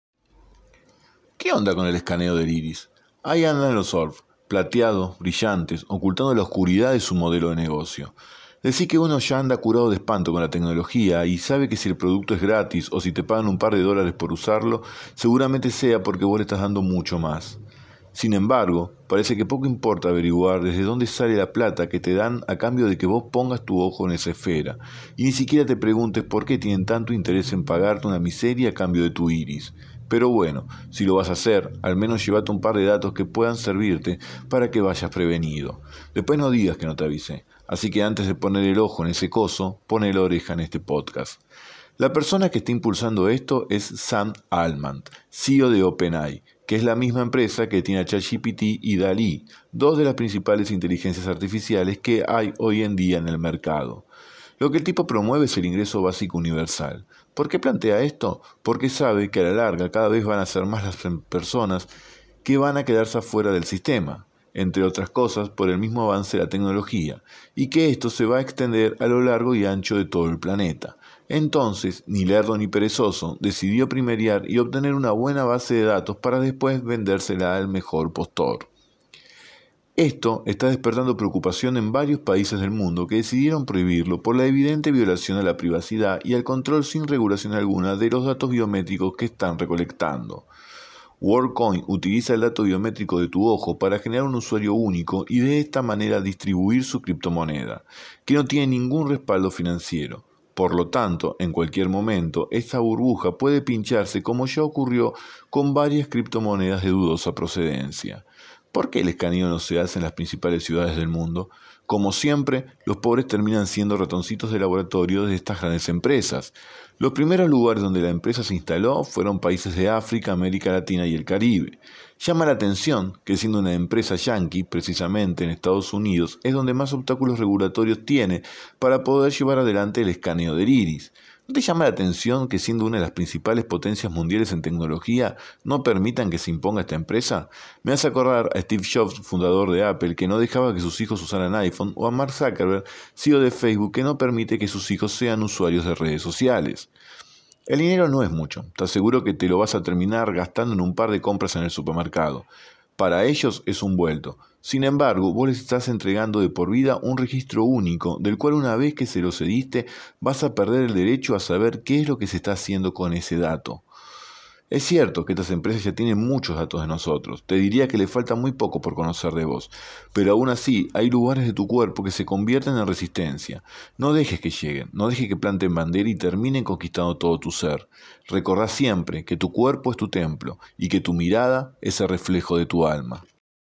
¿QUÉ ONDA CON EL ESCANEO DEL IRIS?
El escaneo del iris es la tendencia de este verano en varios paises del mundo, en este podcast de Multiviral te cuento de que se trata y que cosas deberias tener en cuenta.

Mariano Quiroga, te explica de que se trata. Es director de Multiviral👇💻📱